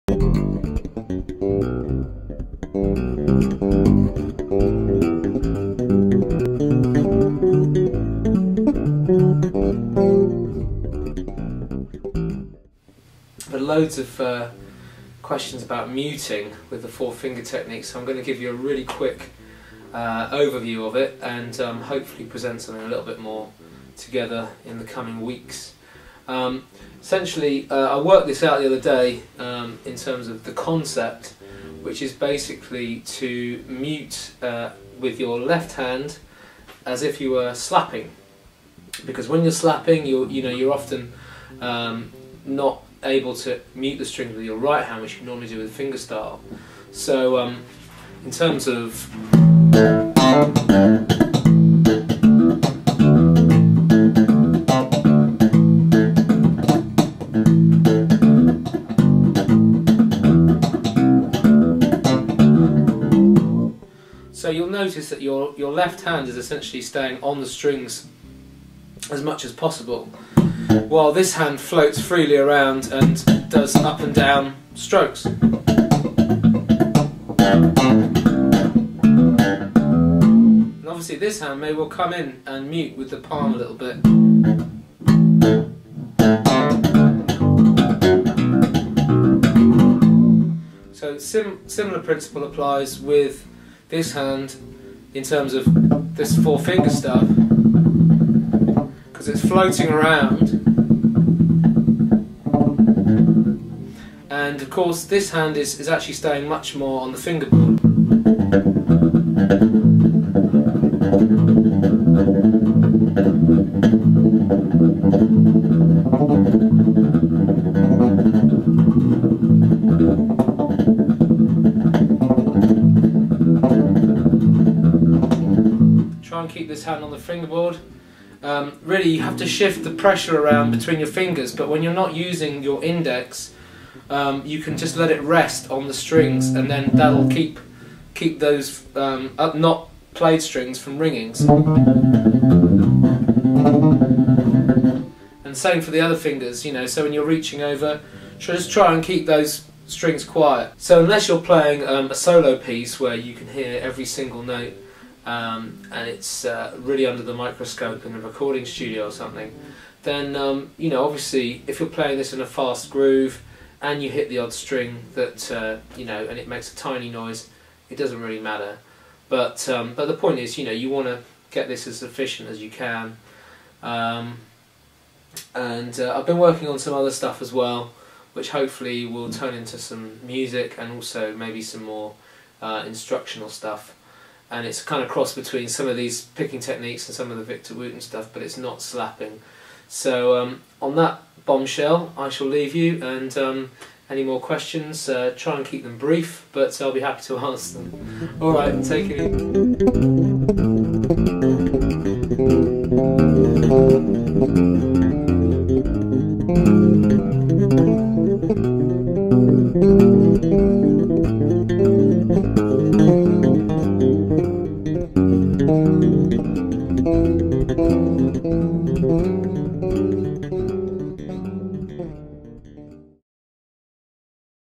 Four Finger Picking On Bass Guitar - Muting Lesson
A really quick look at how to mute when using the Matt Garrison 4 finger picking technique - see more cool music stuff